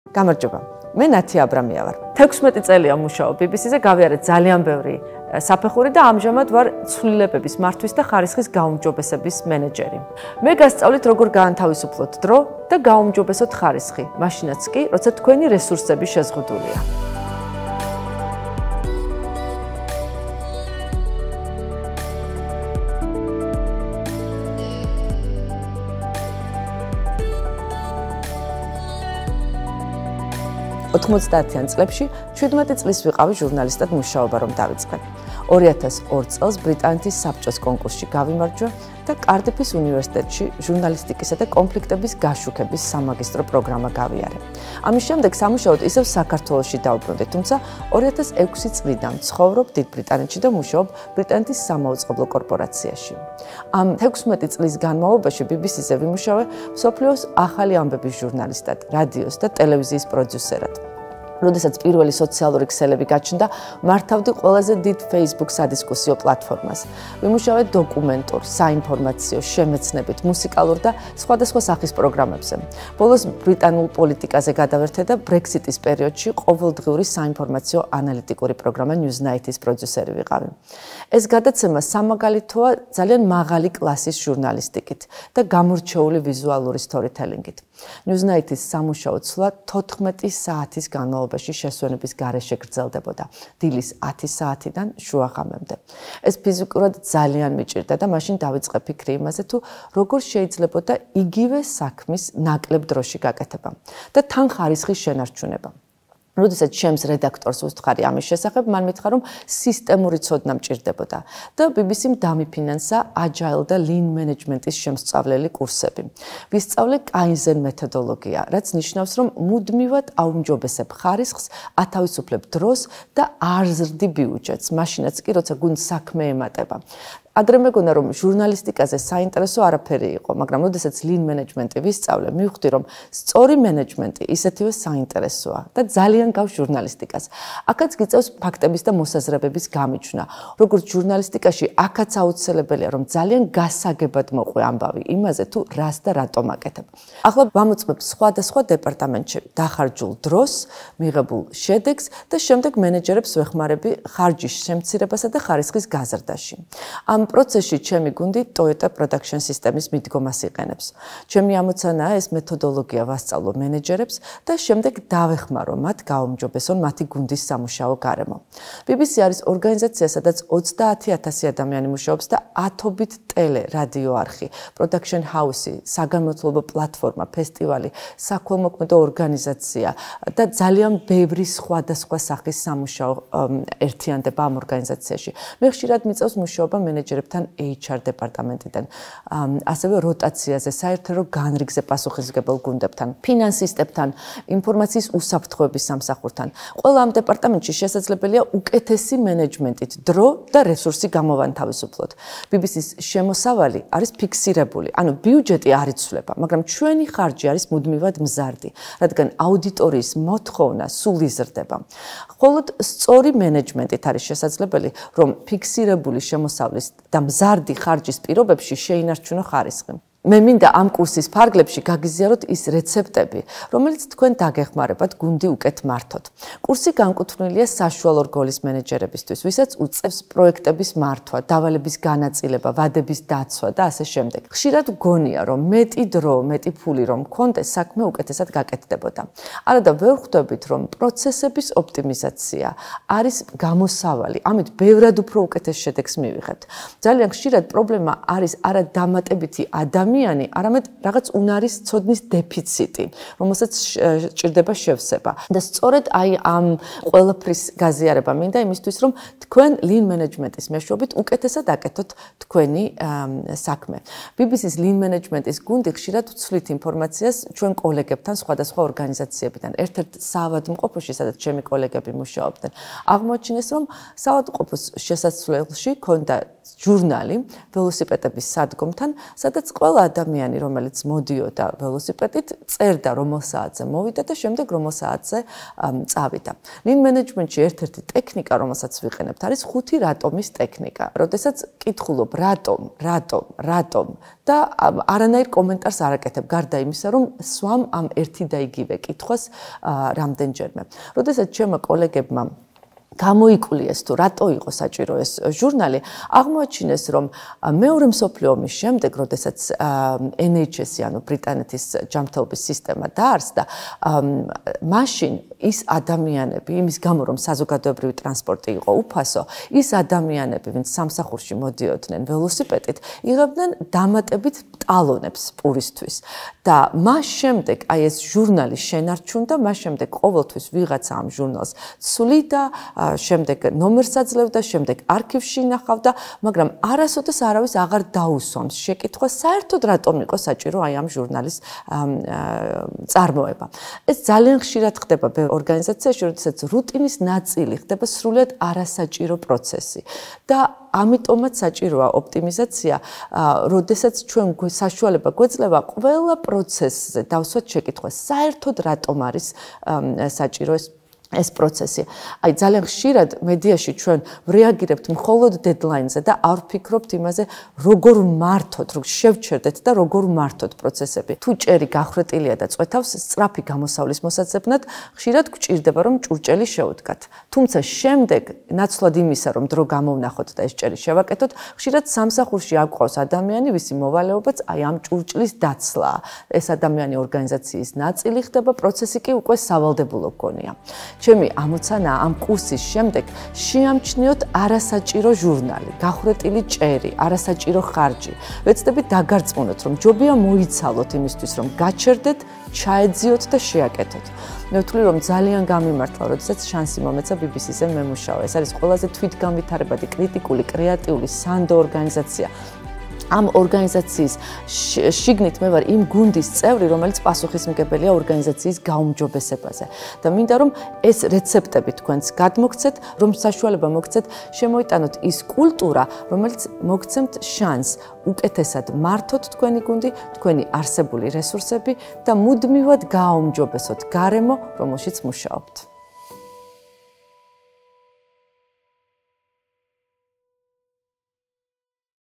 ნათია აბრამიას მასტერკლასი | 1-ლი ლექცია
ყველას, ვინც ჟურნალისტიკითაა დაინტერესებული და სურვილი აქვს იცოდეს, როგორ იქმნება მაღალი ხარისხის მედიაპროდუქტი, მედიააკადემია აძლევს შესაძლებლობას უფასოდ, ონლაინ რეჟიმში, პროფესიონალი ჟურნალისტების მასტერკლასებით მიიღოს ცოდნა.

მედიააკადემიის მთავარი ამოცანა ქვეყანაში მედიაწიგნიერების განვითარების ხელშეწყობაა.

Natia Abramia, Change Management and Quality Improvement Manager at the British Broadcasting Corporation (BBC), shares her 16 years experience at BBC on how to manage media projects and creative teams with limited resources to improve quality and save time.